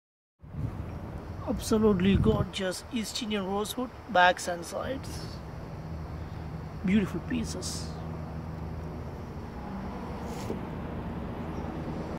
East indian rosewood backs and sides
East indian rosewood backs and sides Excellent quality Exhibition grade in stock
Pls let us know if you need any tonewoods for your guitars
All kinds of exotic tonewoods in wholesale prices
 Regards
California luthier supplies
USA